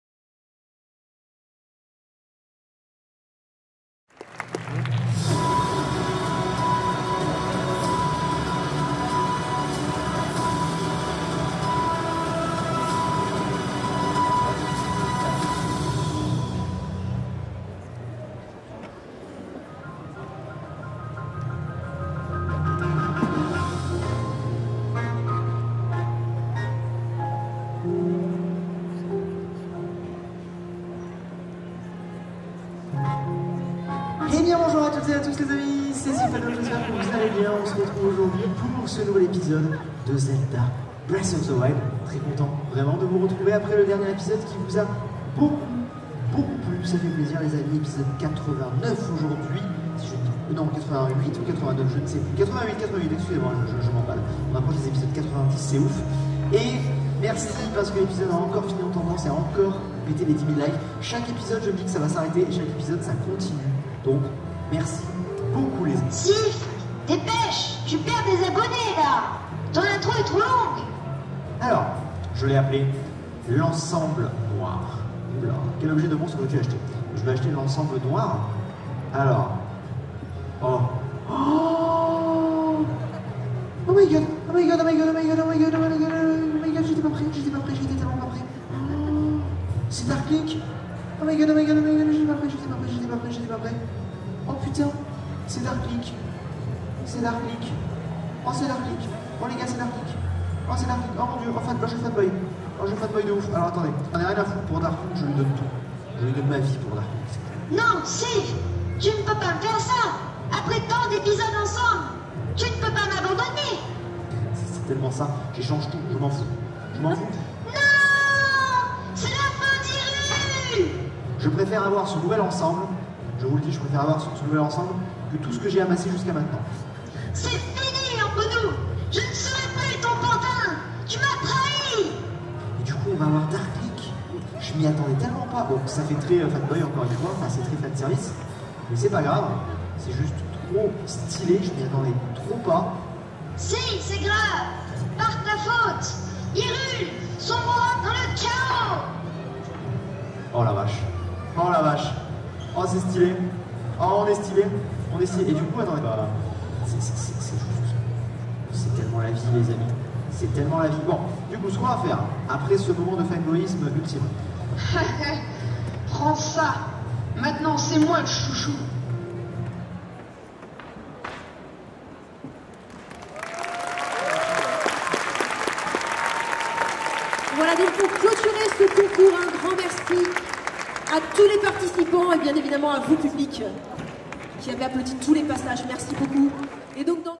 LGS 2019 - Cosplay contest - (Zelda - Link & Dark Link)
Lyon Game Show (LGS) 2019  (Lyon // France), CICAF and CFC French Preliminaries, Solo's and Groups!